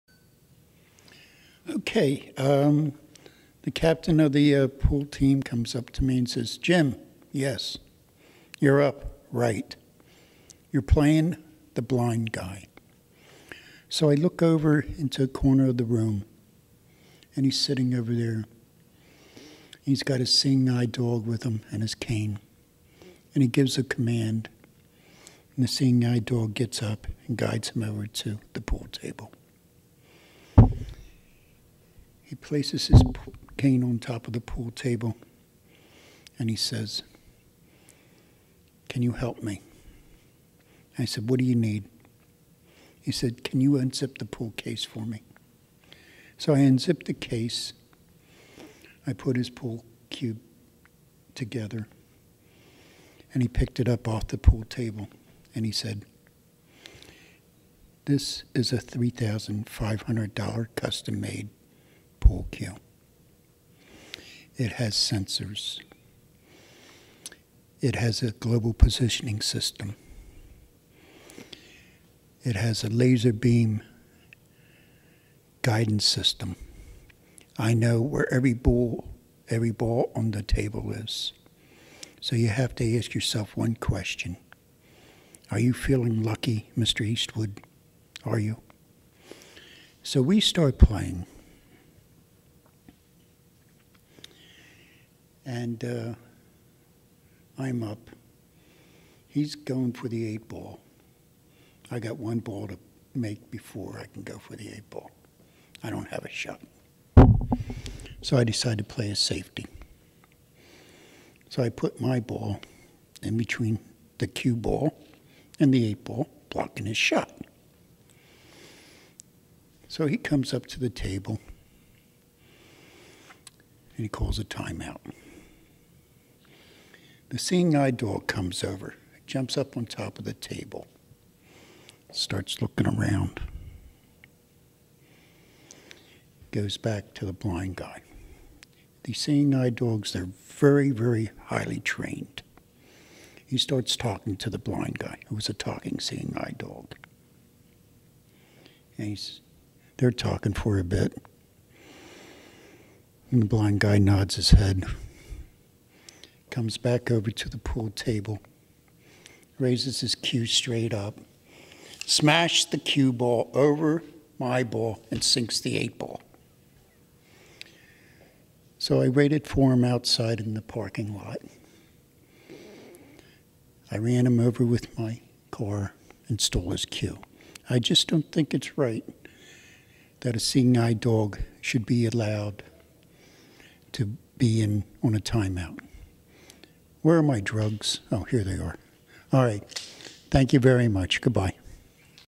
Playing Pool with the Blind Guy
A  comedy  video of being hustled playing pool, and what happened next. His seeing eye dog advised him on how to make the winning shot. It was a talking seeing eye dog. These animals are very intelligent, and highly trained, including teaching pool.